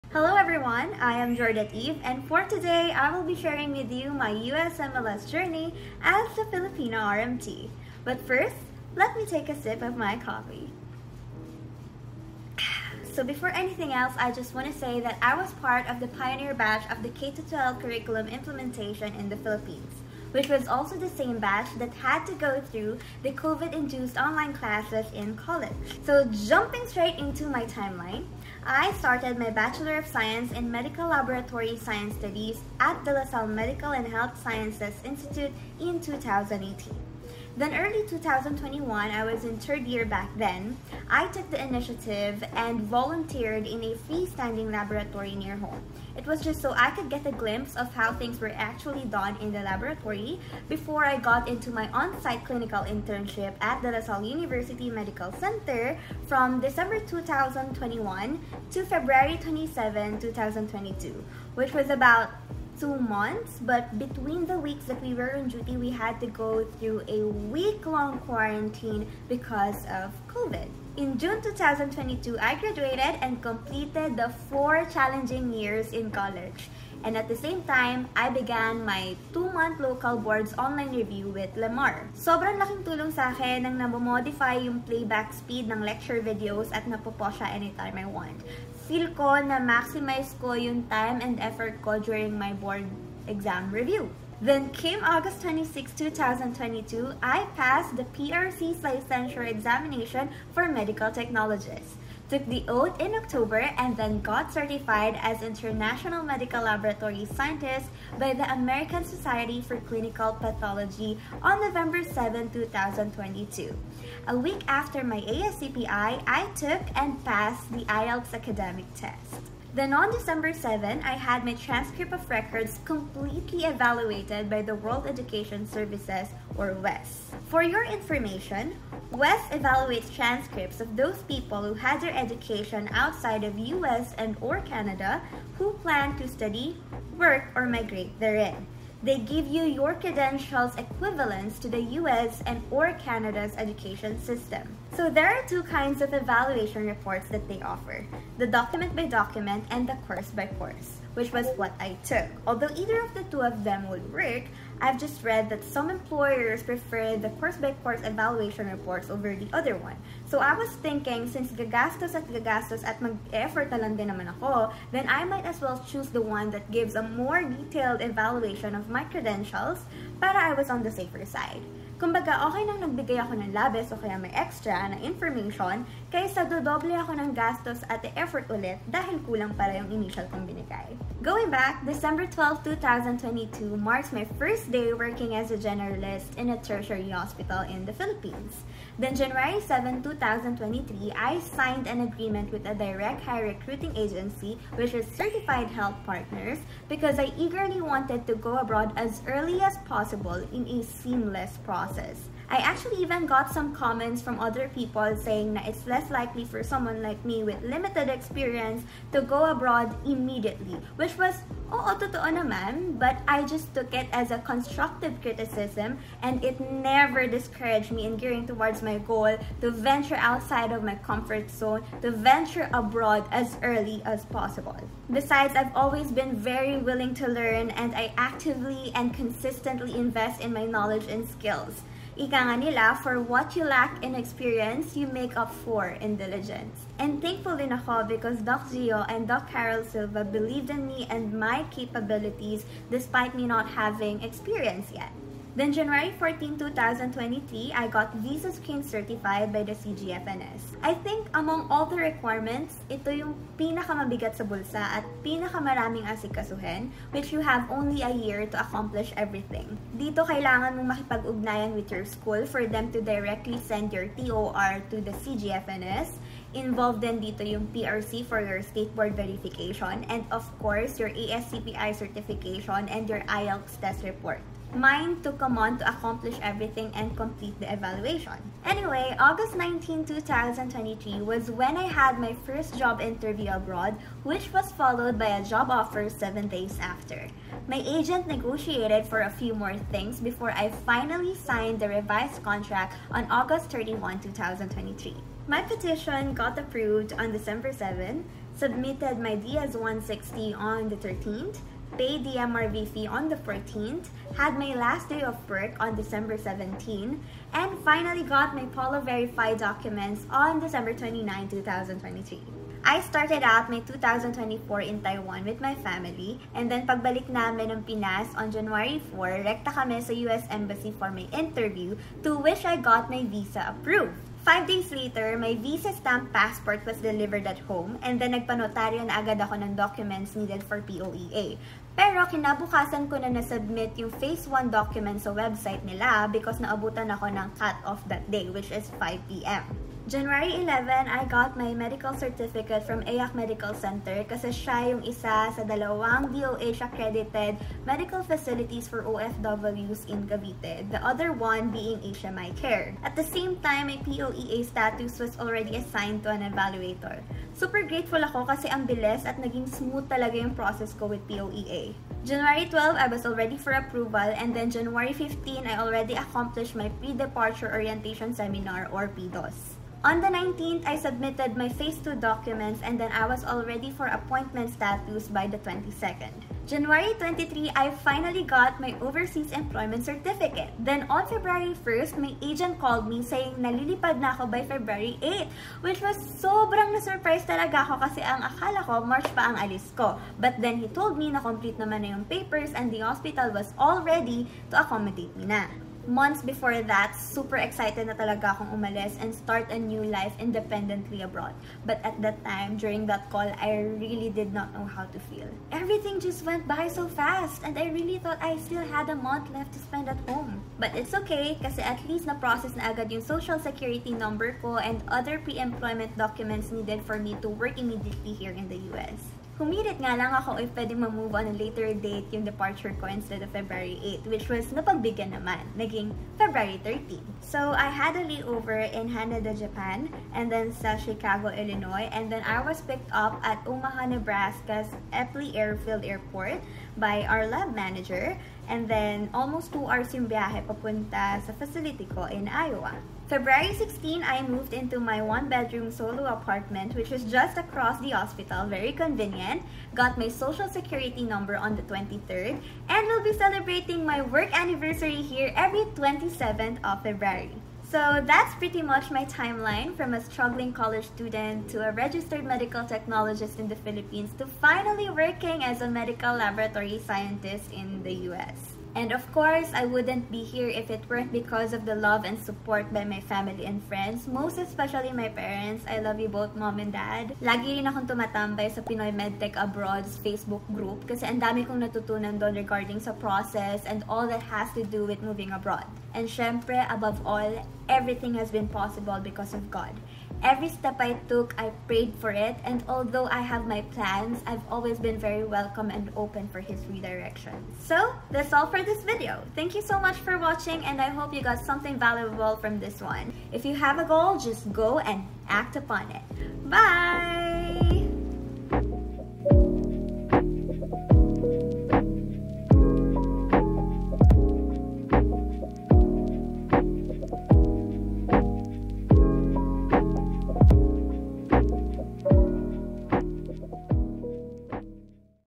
How I Became a Medical Laboratory Scientist in the USA
here's my timeline from being a college student, passing the local boards, working in the PH, and finally practicing medical laboratory science in the US ❤